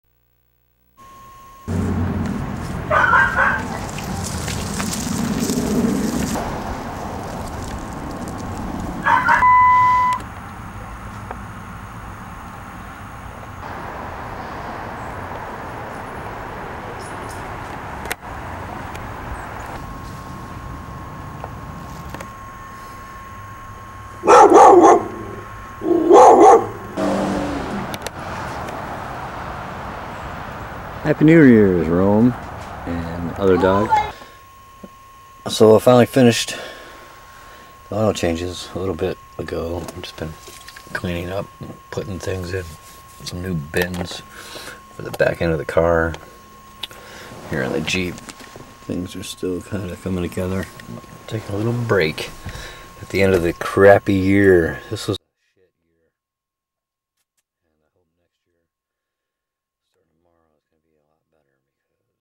RDMVTL 9225 ~ 12/31/2006 {MY JOB/STORY} OIL CHANGES & END OF YEAR REFLECTIONS @ {JOYLAND} DURHAM ,NC
DURHAM , NORTH CAROLINA ~ SUNDAY 12:40 PM (EST) Changing the oil in both the JEEP & the Ford Escort Plus my views & reflections from year's end 2006.
(G)